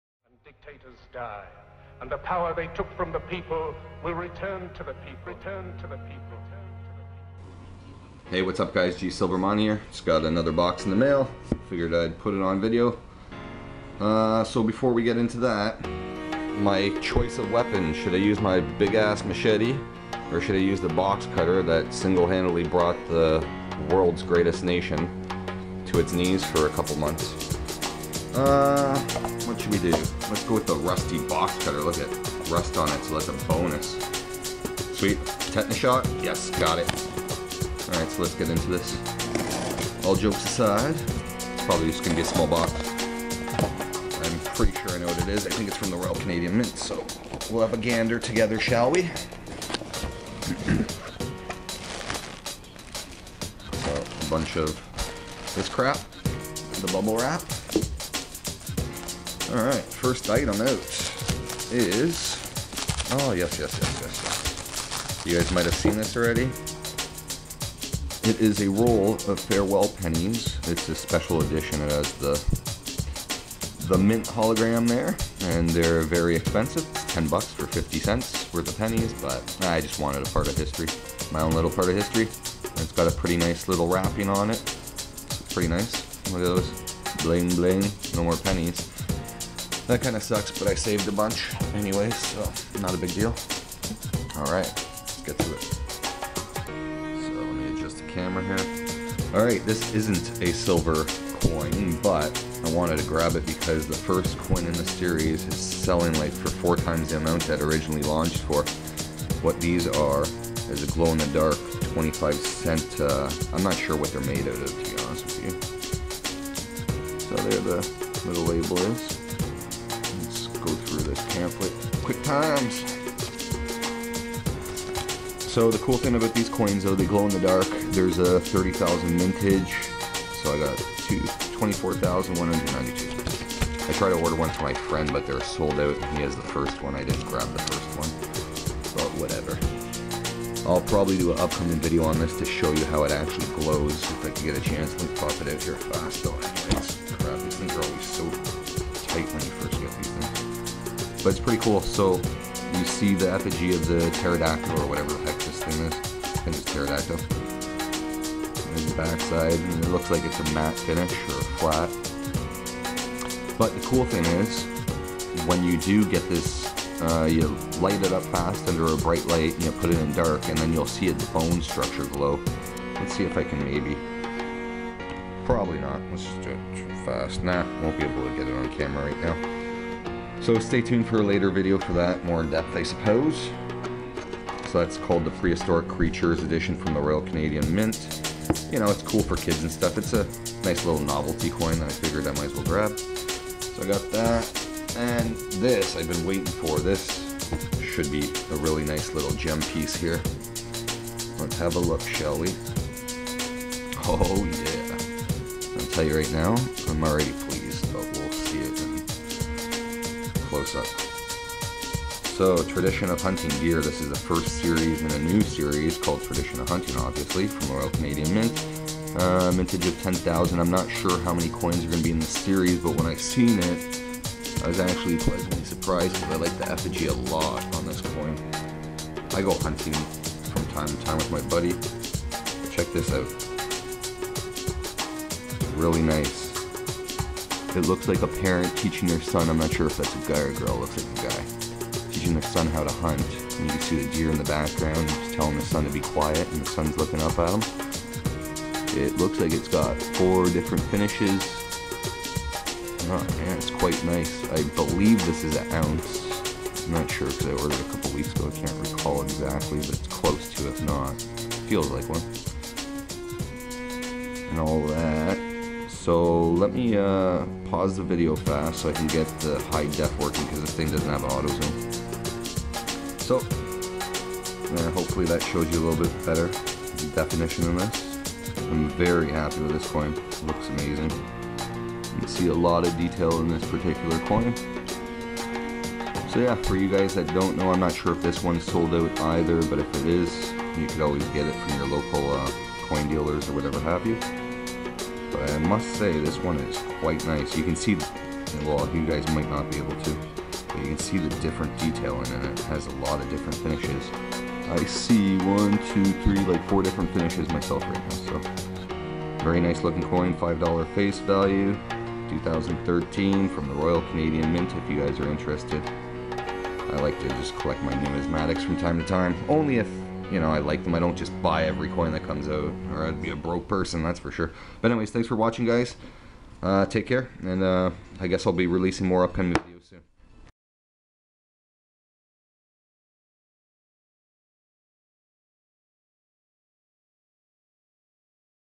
Tradition of Hunting SILVER coin plus glow in the dark
Just got my glow in the dark coin 2nd in the series, SILVER tradition of hunting deer coin and more...